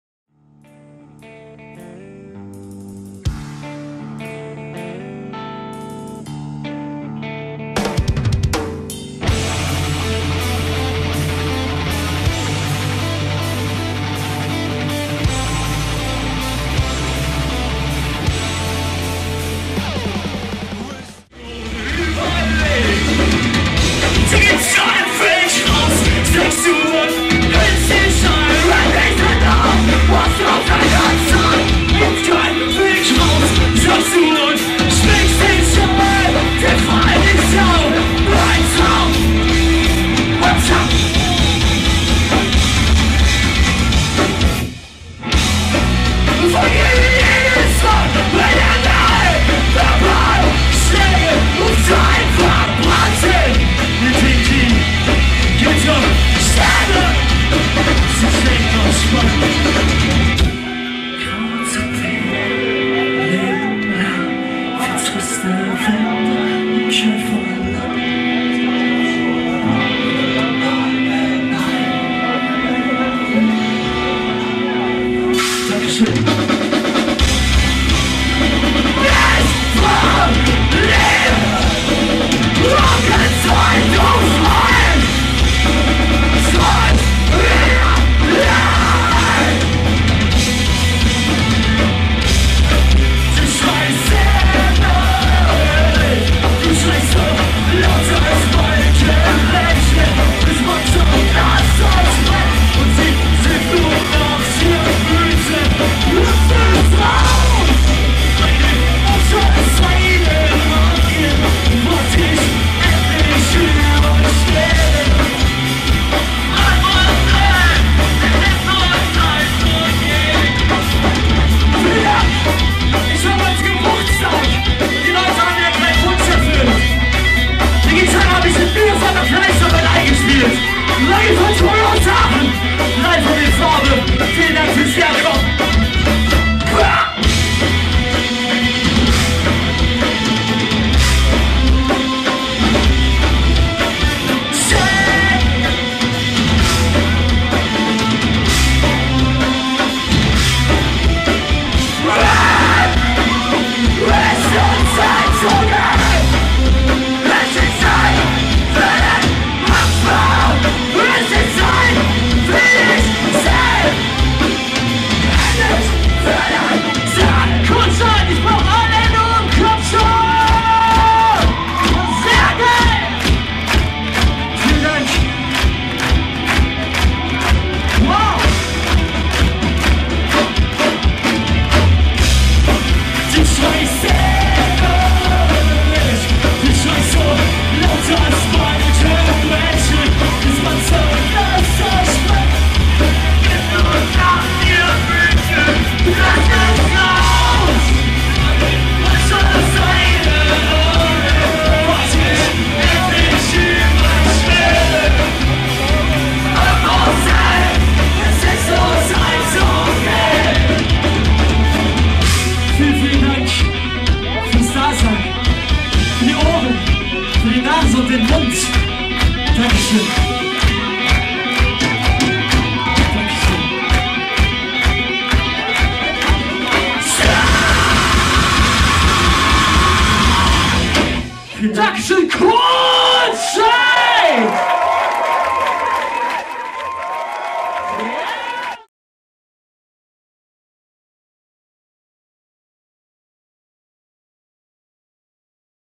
20.01.2011 - Kohlscheid - Longing For Tomorrow - special
Longing For Tomorrow - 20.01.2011 - Kohlscheid - special show with david as a free singer - NEW YEARS METAL